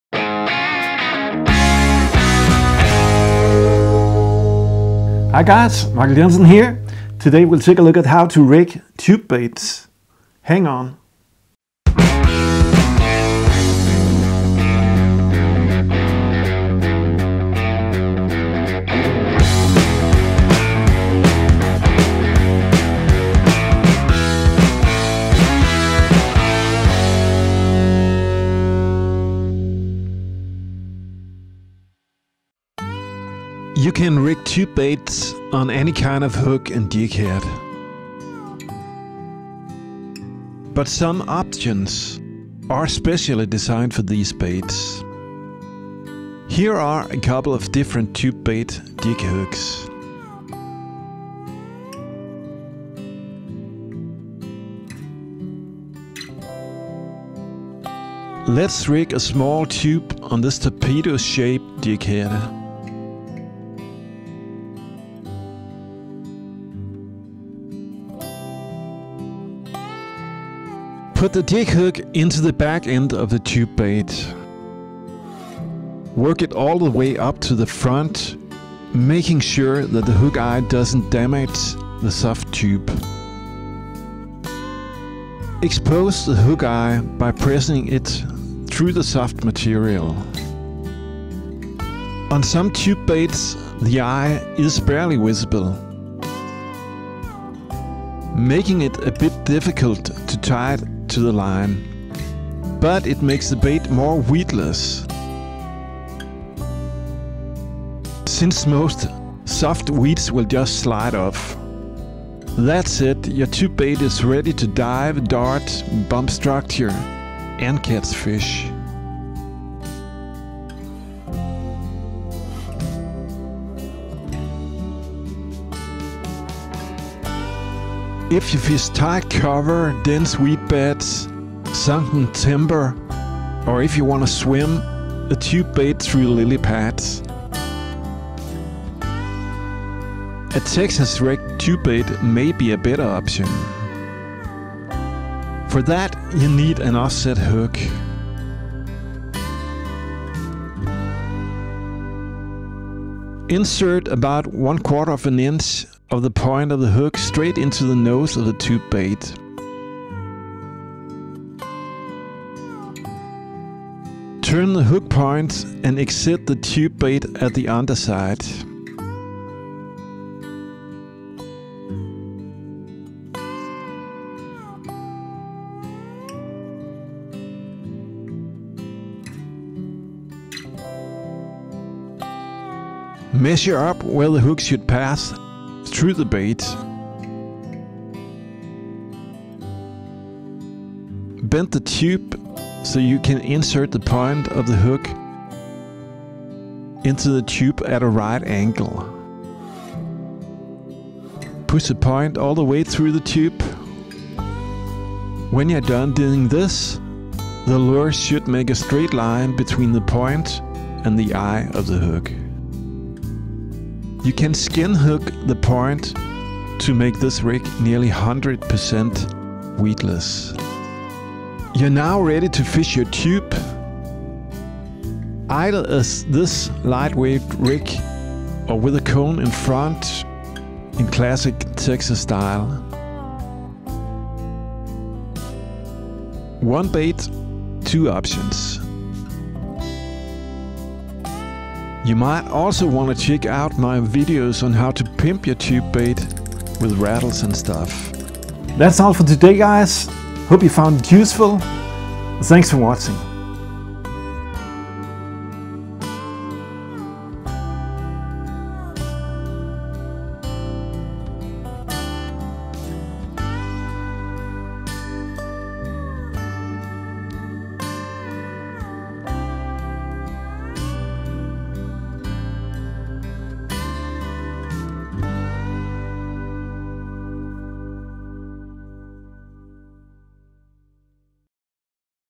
How to rig Tube Baits - on jig head - and Texas style
Rigging tips and underwater swimming action. Tube baits are great for imitating crawdads, squids, shrimps, leeches, eels and other critters - and they are dependable producers when targeting bass, pike, perch, walleye, trout and most other predators. They work great for anything from hammering the bottom to lightline finesse techniques. Learn how to rig these cool baits on a jig head and on a Texas style weedless rig. Meet the new season with these tips in your back back :0)

And tube bait jig heads on this